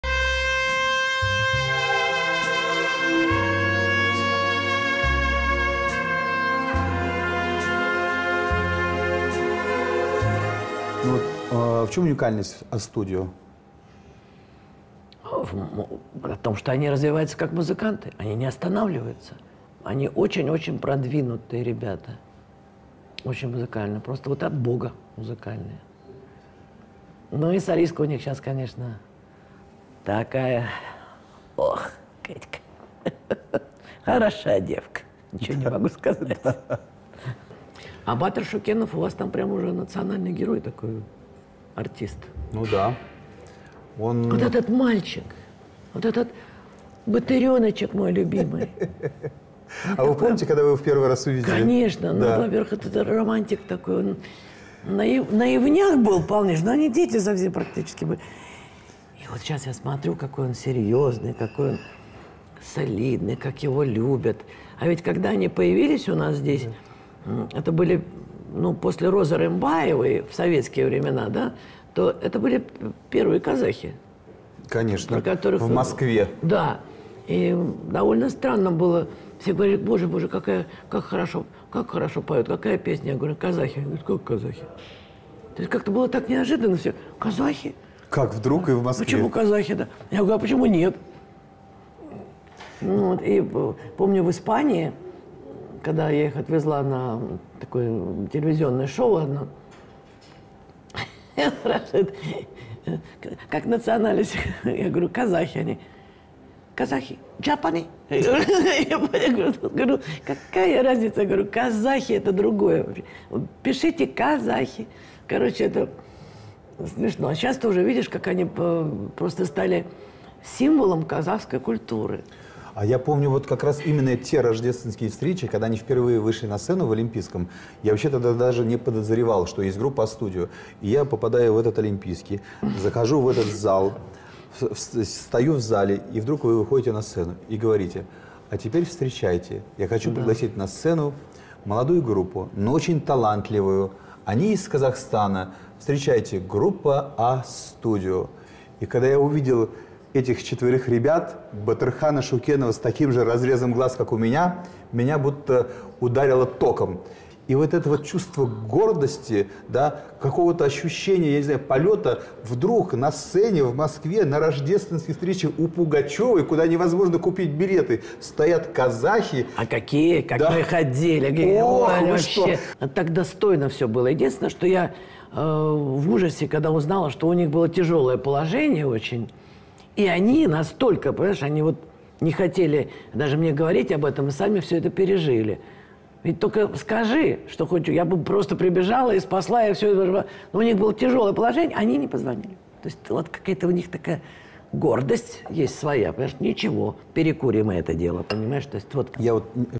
Алла: Только скажи✨Я бы быстро прибежала и спасла, и всё сделала✨Продолжение интервью Аллы для МузТВ
БУДЕТ ИНТЕРЕСНО✨НОВАЯ ГРУППА для тех, КТО ЛЮБИТ АЛЛУ ---

Гарри и Лизонька.
Сторис и Инстаграм.

Контент канала не является авторским и коммерческим.